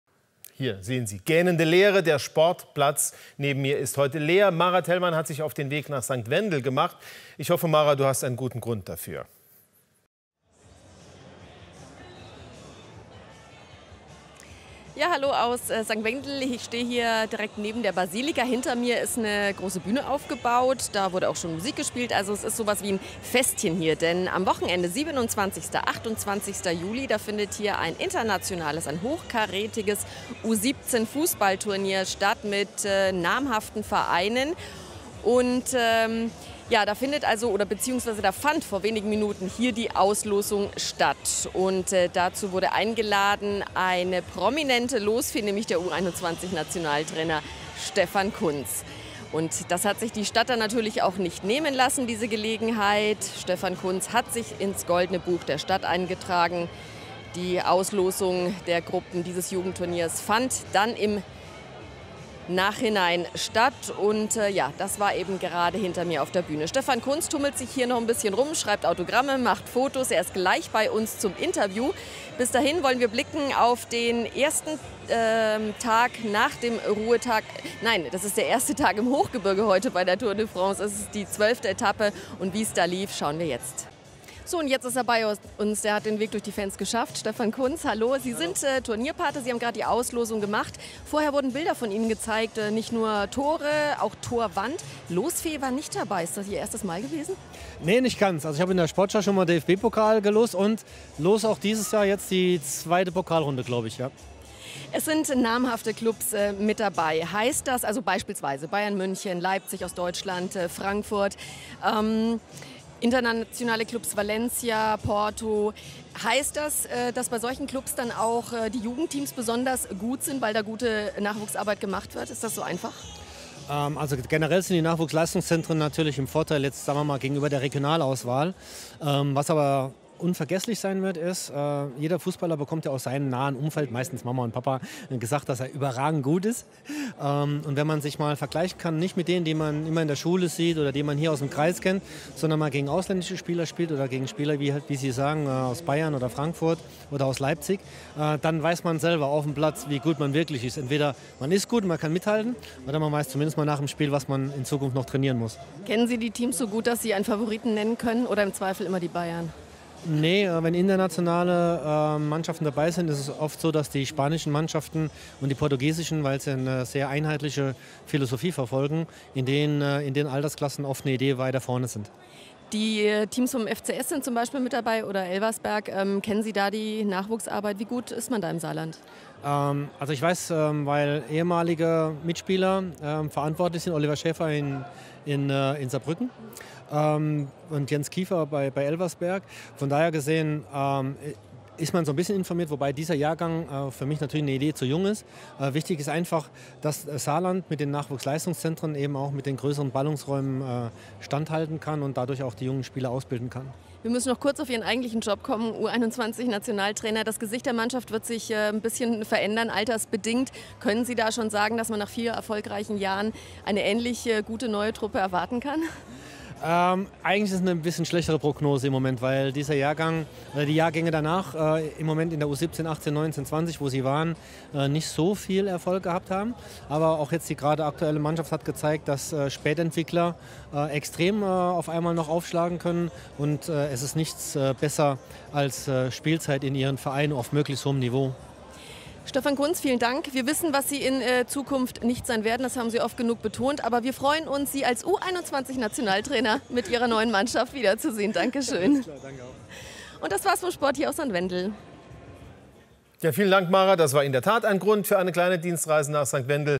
SR aktueller Bericht - Vorbericht EuroYouthCup mit Stefan Kuntz
Vorbericht zum Besuch und Gruppenauslosung des SR mit dem seitens Presenter Kreisstadt St. Wendel und Turnierideenvater Michael Schultheis vermittelten Turnierpaten Stefan Kuntz, DFB U21 Trainer: +++ zum Spielplan
🔥Die besten Talente Europas kurz bevor sie Profis werden zum Anfassen, "nur" bei der
U17 Champions League in der Kreisstadt St. Wendel – Bank1Saar EuroYouthCup powered by
Sport Bies 🏆 am 2️⃣7️⃣ & 2️⃣8️⃣ Juli in der
⚽ EMI-Arena Am Sportzentrum St. Wendel ⚽ 📣 mit:
⚽ FC Bayern München
⚽ FC Valencia
⚽ Galatasaray Istanbul
⚽ FC Porto
⚽ Eintracht Frankfurt
⚽ RB Leipzig
⚽ Hertha BSC Berlin
⚽ 1. FC Saarbrücken
⚽ SV Elversberg
⚽ Telekomshop St. Wendel Regionalauswahl